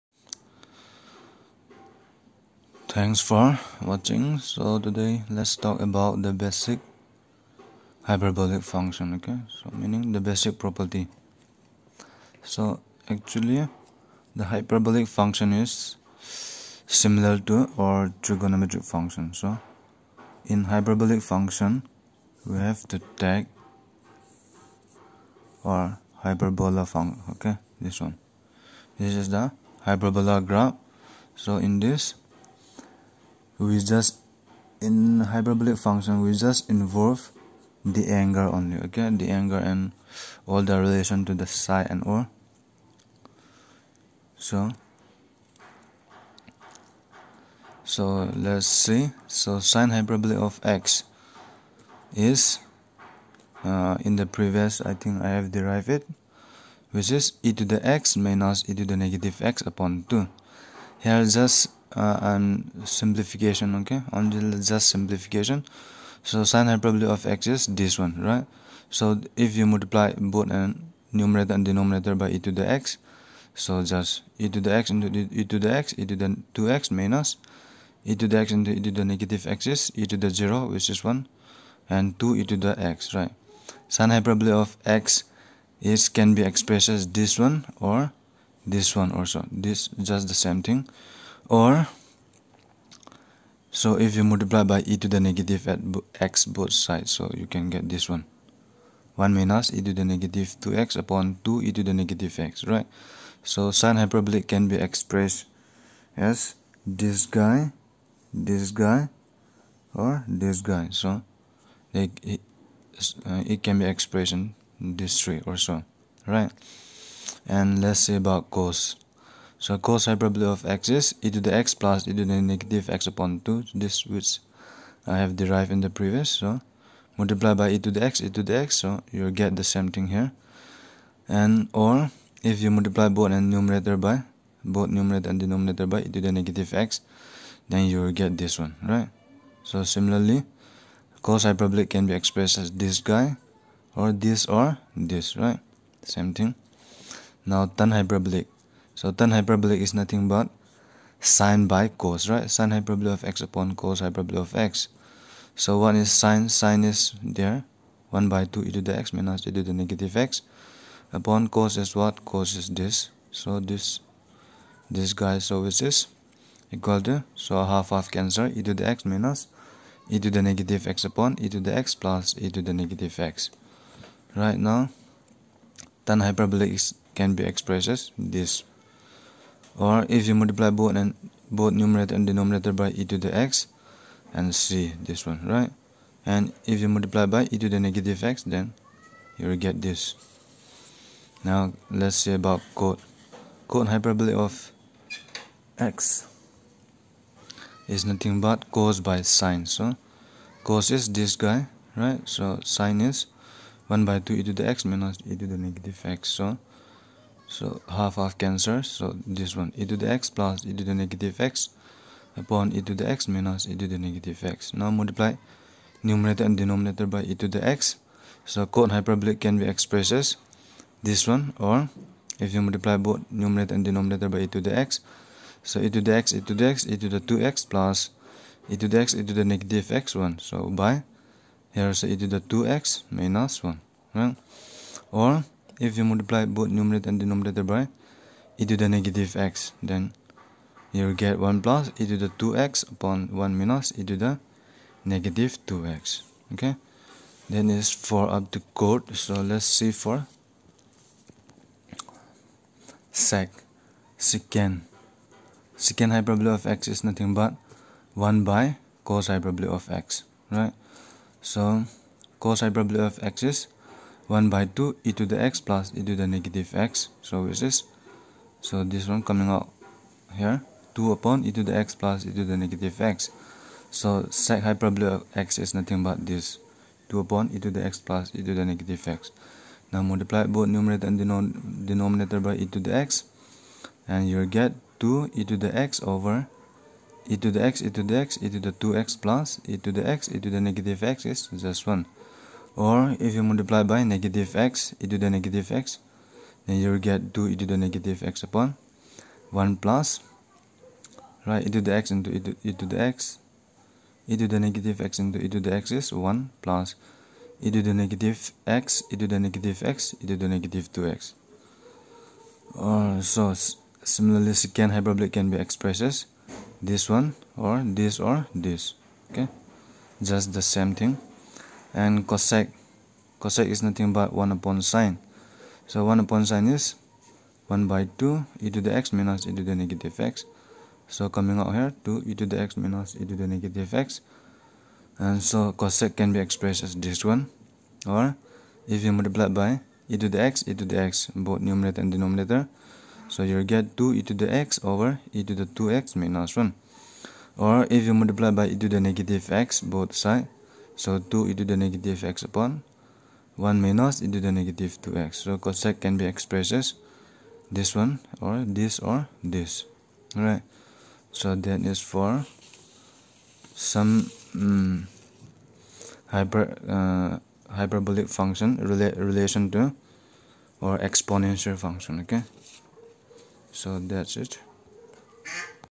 THE BASIC HYPERBOLIC FUNCTION
Today, we are going to discuss the basic hyperbolic function or basic definition of hyperbolic function in term of exponential function.